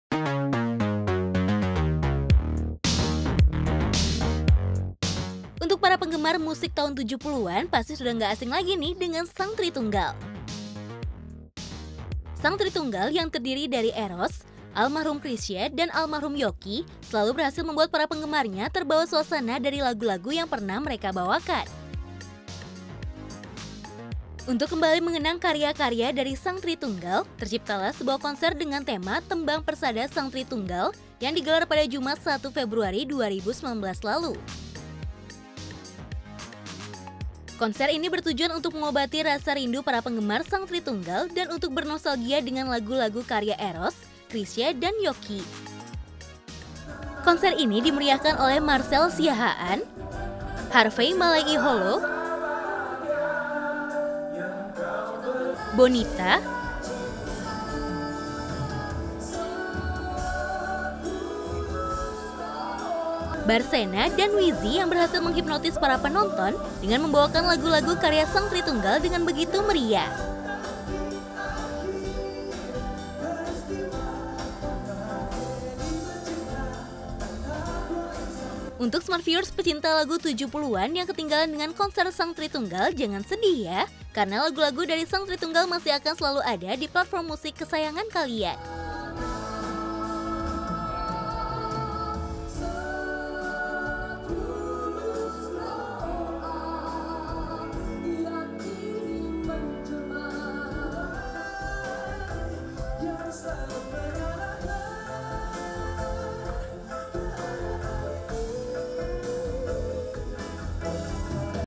Konser Tembang Persada Sang Tritunggal Berhasil Menghipnotis Para Penonton | ZOOM IN
Untuk kembali mengenang karya-karya dari Sang Tritunggal, terciptalah sebuah konser dengan tema Tembang Persada Sang Tritunggal, yang digelar pada Jumat 1 Februari 2019 lalu. Konser ini bertujuan untuk mengobati rasa rindu para penggemar sang tritunggal, dan untuk bernostalgia dengan lagu-lagu karya ErrosDjarot, Chrisye dan JockieSoerjoprajogo. Konser ini dimeriahkan oleh Marcell Siahaan, Harvey Malaihollo, Bonita, Barsena, dan Wizzy, yang berhasil menghipnotis para penonton dengan membawakan lagu-lagu karya Sang Tritunggal dengan begitu meriah.